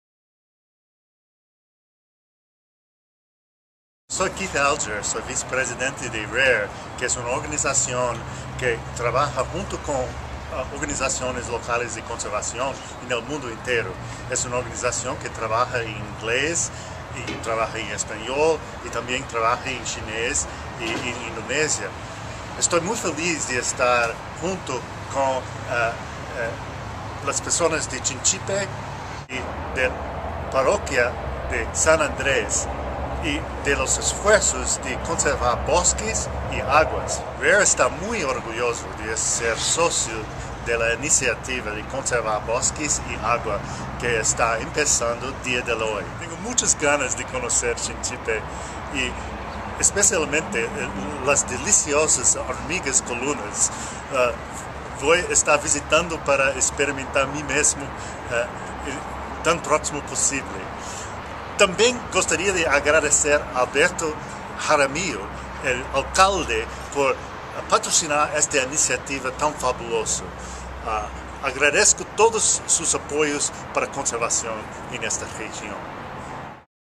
Mensaje motivador de Keith Alger, Vicepresidente Rare Latinoamérica
Mensaje motivador de Keith Alger, Vicepresidente Rare Latinoamérica para el alcalde de Chinchipe, Ecuador / Words of motivation from LA VP Keith Alger to the mayor of AZE campaign site in Ecuador